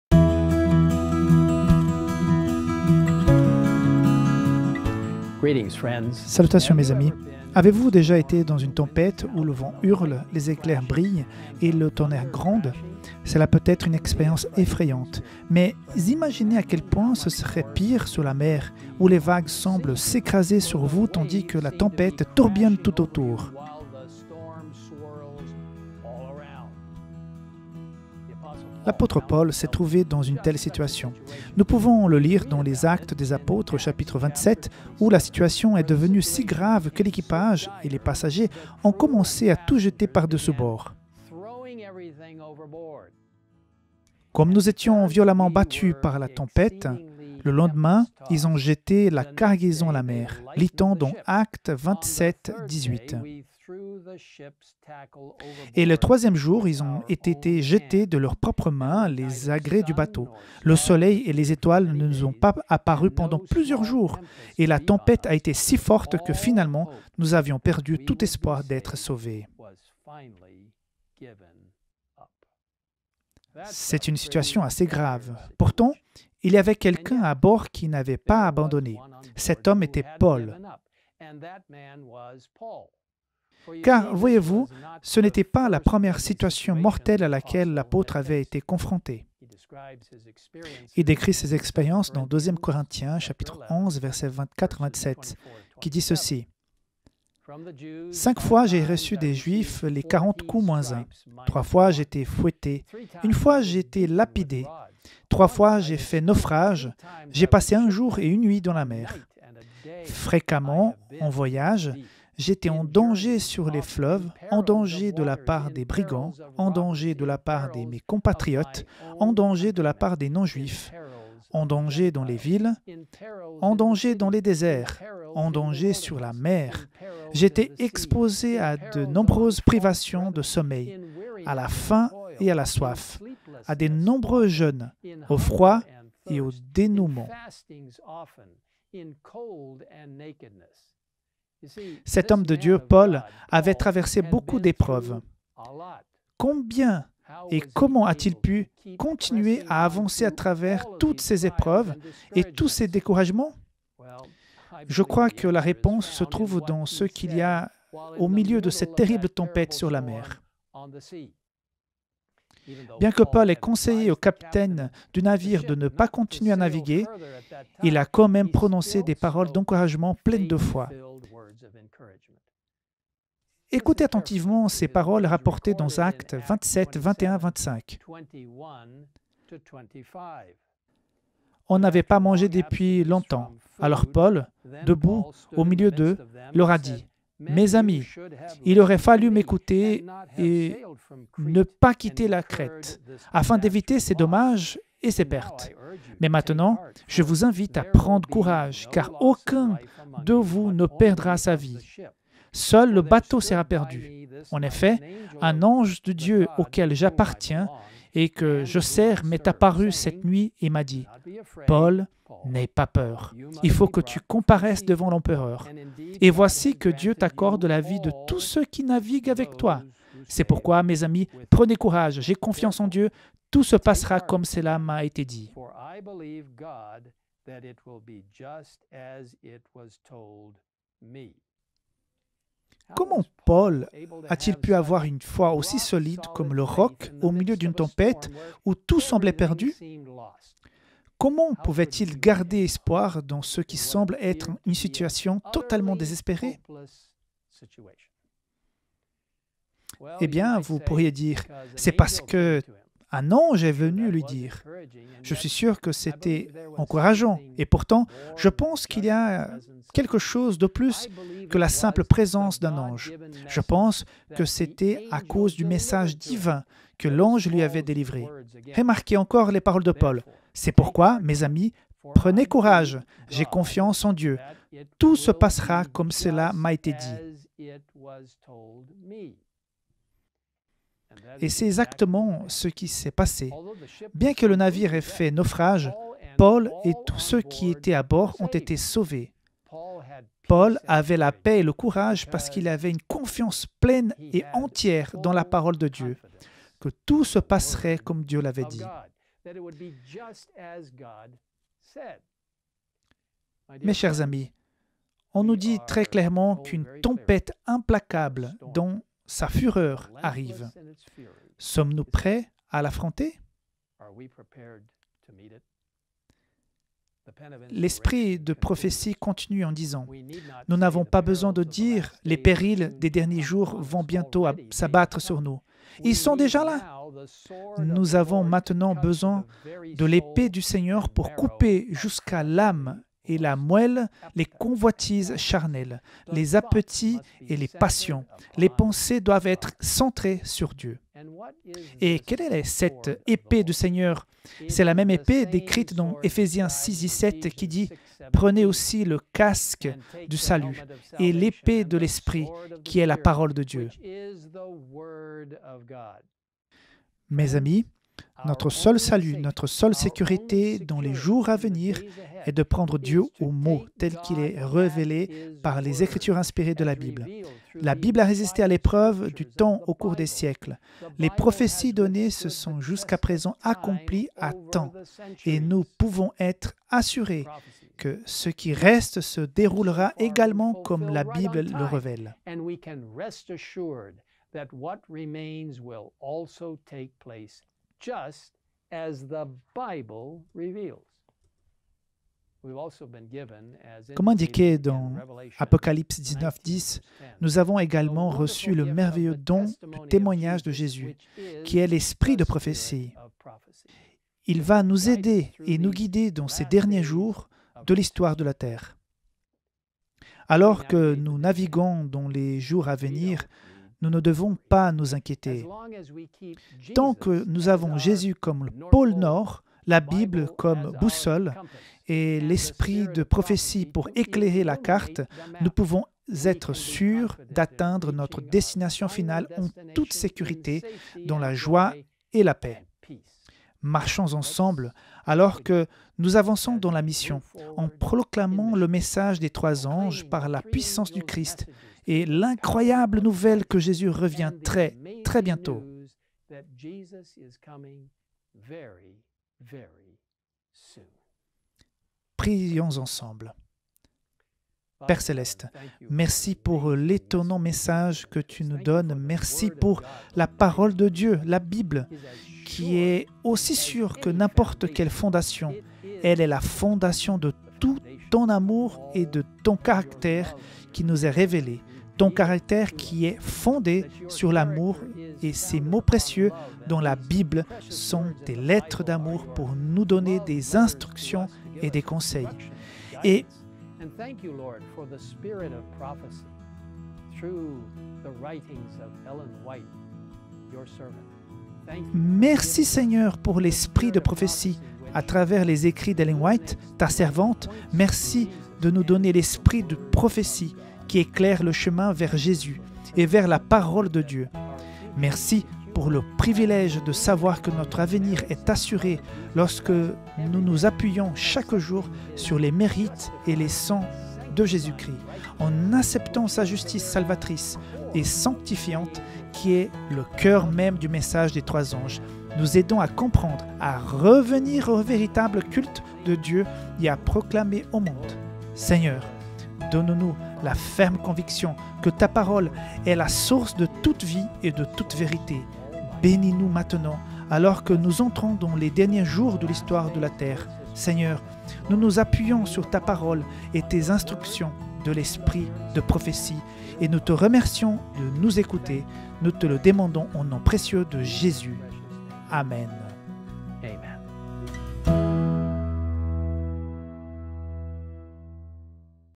Message spécial du Pasteur Ted Wilson, 16 Juillet 2021, Surmonter la tempête de la fin des temps
Comment pouvons-nous avoir une foi solide comme un roc au milieu d'une tempête alors que tout semble perdu ? Comment pouvons-nous garder espoir dans ce qui semble être une situation totalement désespérée ? Découvrez-le dans le message vidéo de cette semaine.